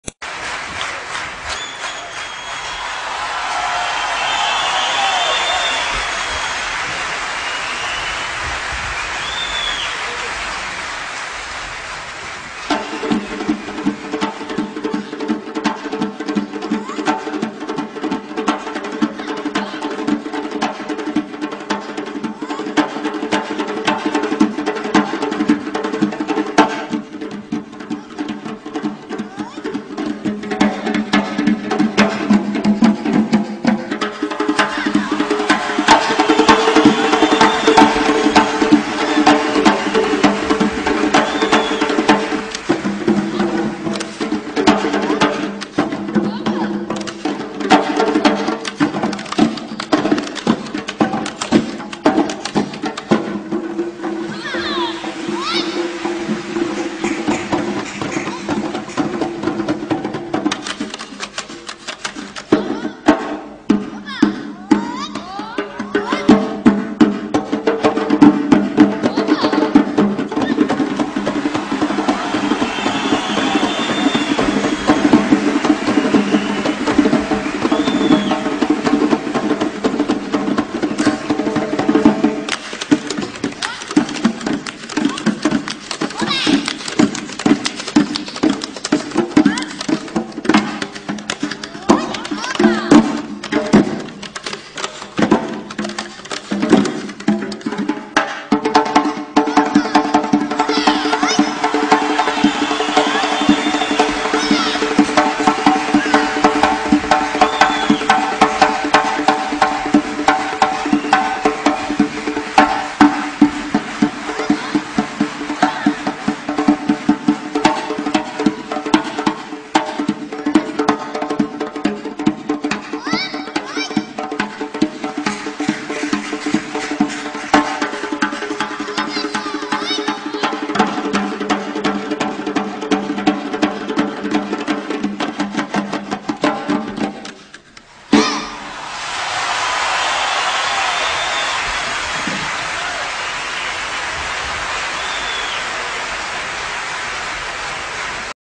Little boys drumming
I got these little drumming boys on Facebook and thought these are too good to be hidden. Amazing coordination and teamwork!!! Brilliant